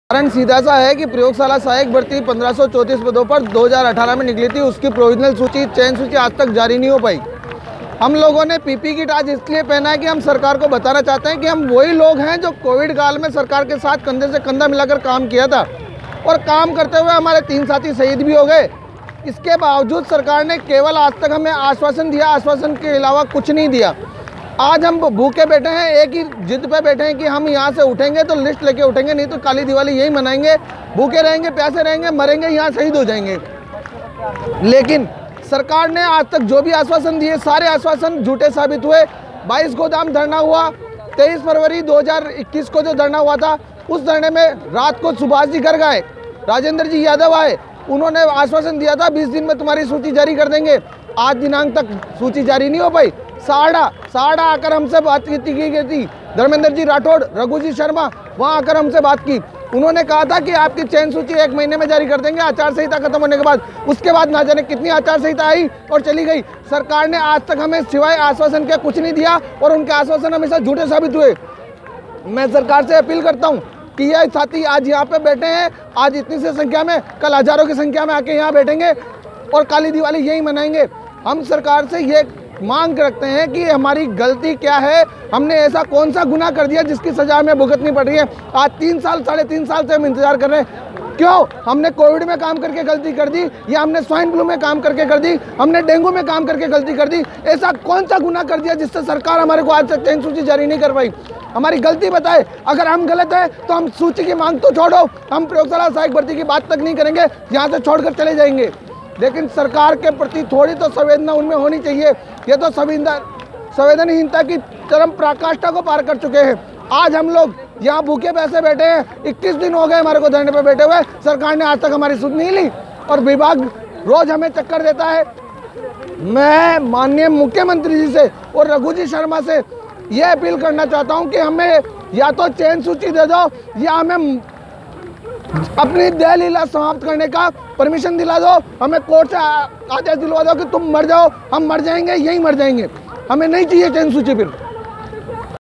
ppe_kit पहन, भूखे प्यासे बैठे कर्मचारी दो दिन तक खाना नहीं खाएंगे:गोविंद नारायण सोनी प्रदेश अध्यक्ष
चिकित्सा विभाग प्रयोगशाला सहायक भर्ती